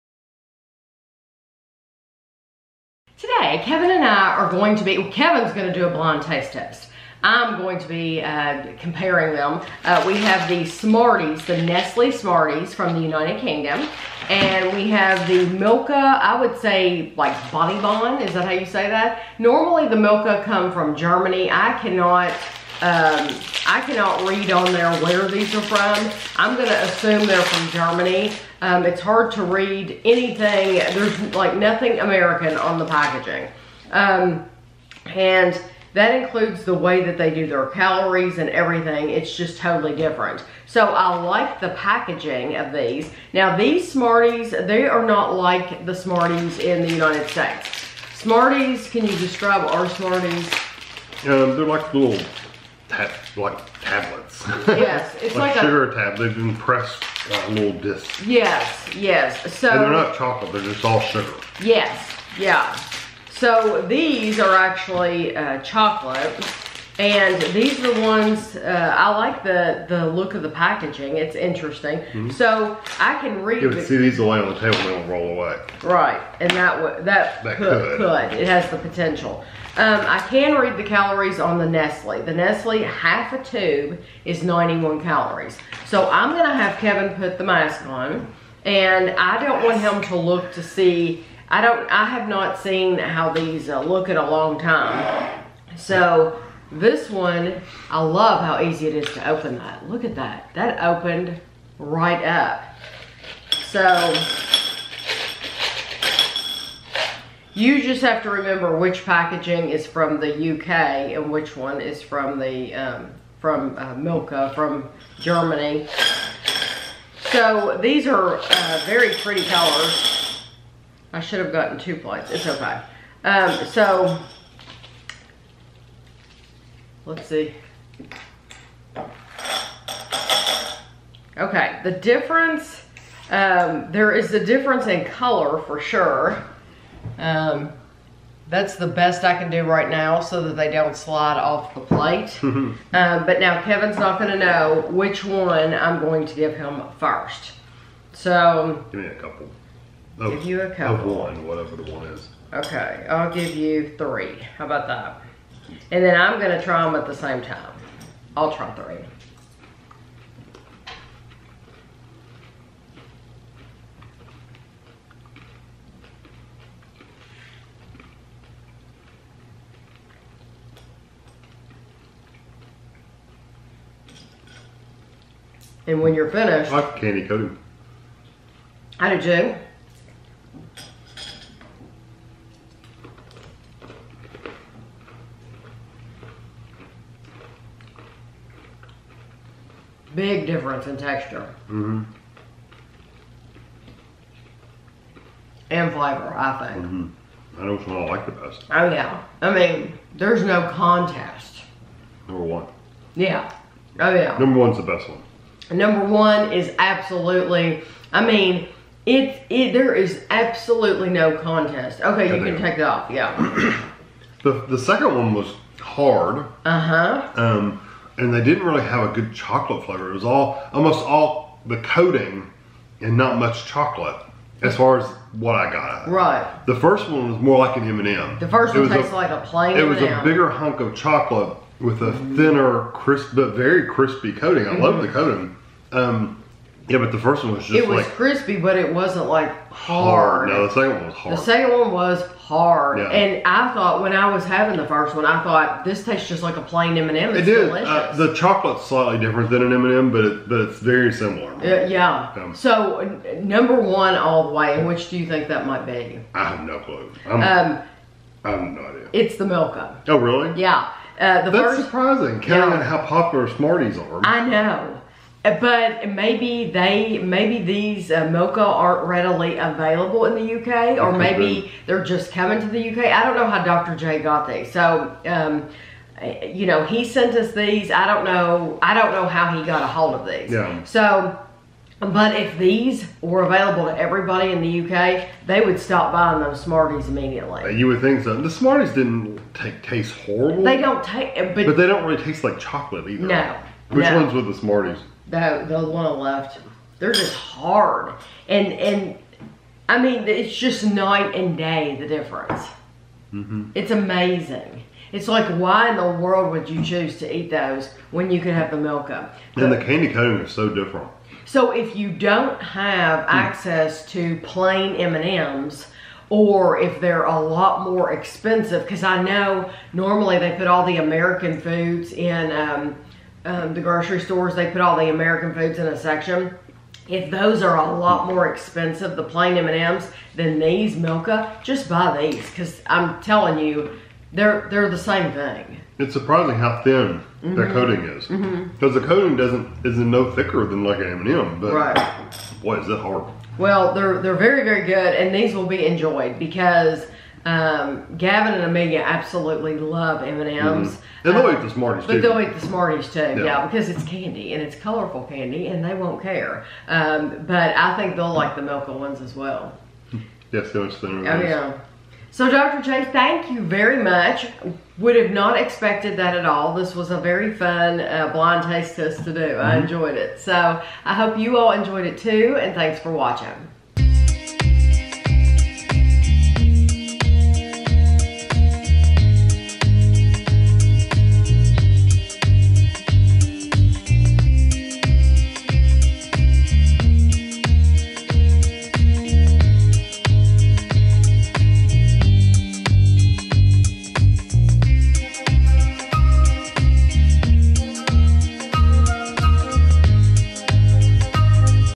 Milka & Bonibon vs Nestle Smarties – Blind Taste Test
In this video we are comparing the Milka & Bonibon to the Nestle Smarties to see which one has the better flavor. They were both mailed to us from Dr J in the UK!